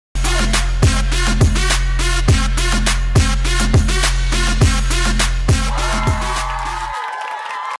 Tutorial VLOG | ETek City Mini Portable Butane Camping Stove
Palm-size portable gas stove, works with any propane cans (shown in the video); perfect for your next on the go adventure.